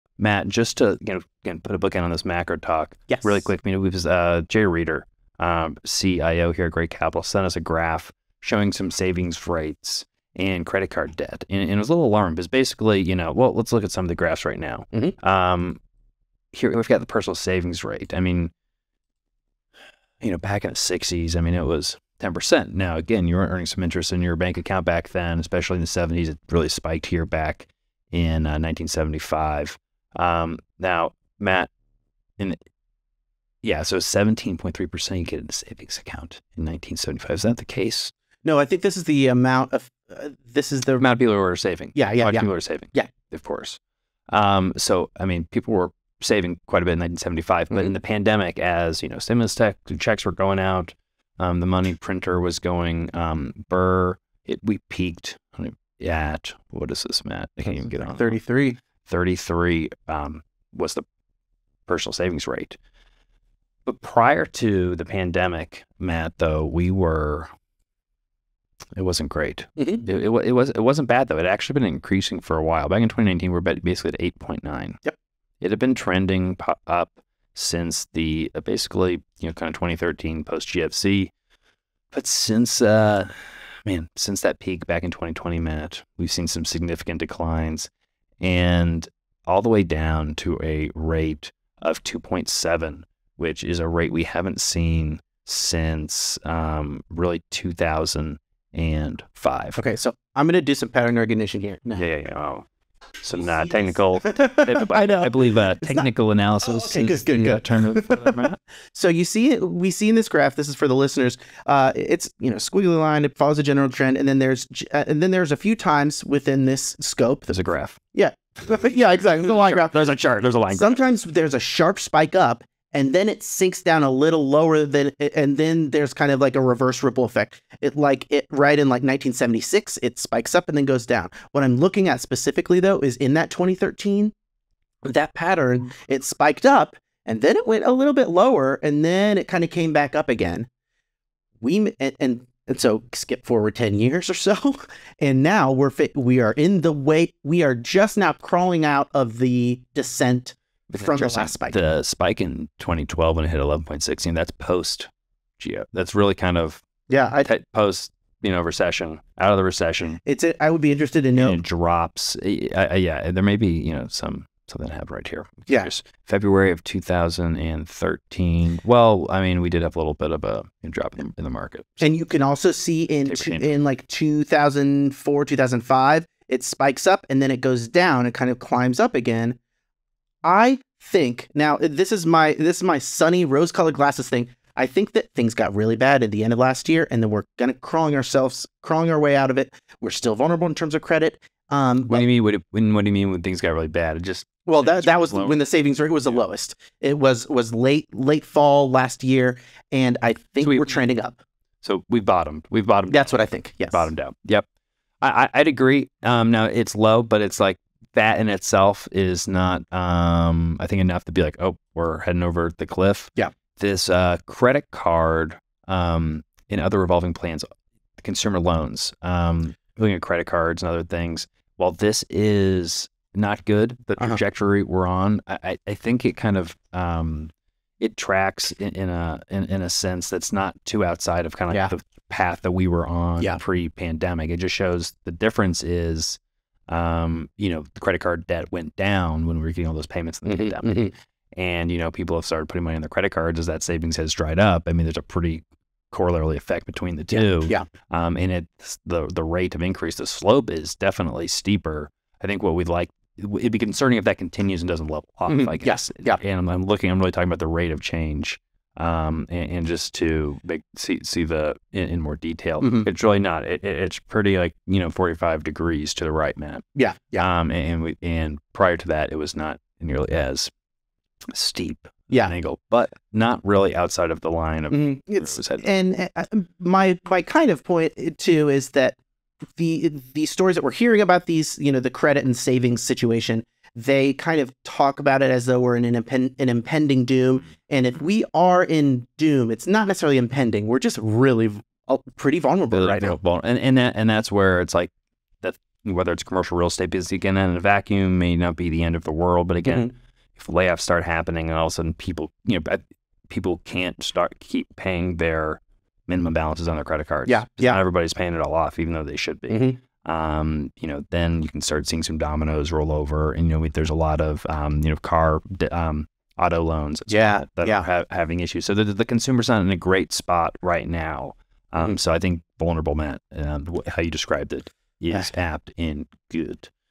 The Reality of Credit Card Debt and the Saving Rate in Q2 2023
In this clip from the most recent episode of the Gray Report, the Gray Capital team discusses the current state of consumer saving and credit card debt.

For the latest multifamily news from across the internet, visit the Gray Report